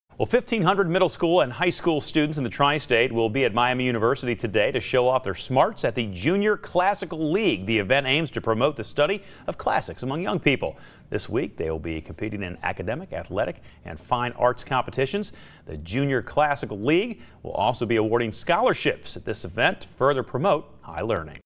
OJCL holds summer convention in Oxford
OXFORD, Ohio (WKRC) - The largest organization in the world dedicated to the study of classical civilization is holding an event in Oxford.

Approximately 1500 middle school and high school students in the Tri-State will be at Miami University to show off their smarts at the national convention for the Junior Classical League.

The event aims to promote the study of classics among young people. This week they will be competing in academic, athletic, and fine arts competitions.

the Junior Classical League will also be awarding scholarships at this event, to further promote high learning.